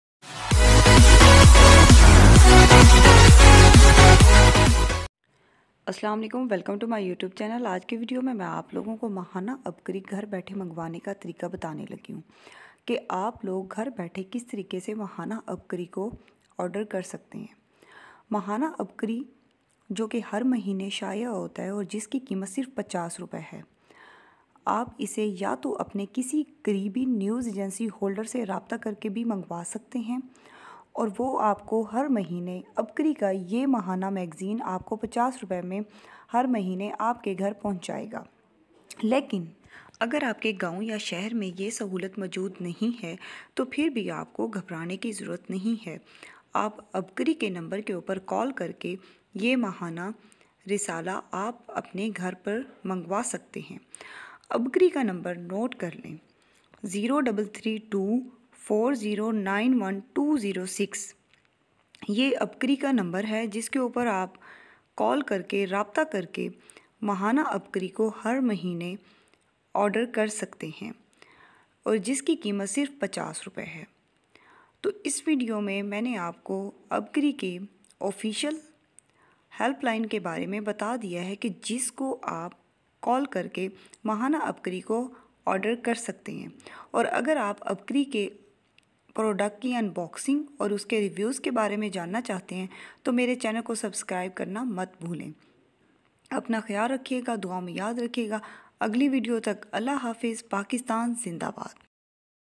Mahana Ubqari Ghar bethy mangwany ka tariqa || Ubqari number k bary main janye
اس ویڈیو میں ماہانہ عبقری رسالہ گھر بیٹھے کے لیے
 کیا کرنا چاہیے کہ بارے میں بتایا جائے گا۔
آ پ عبقری کے دفتر کے نمبر پر فون کر کے ماہانہ عبقری گھر بیٹھے منگوا سکتے ہیں۔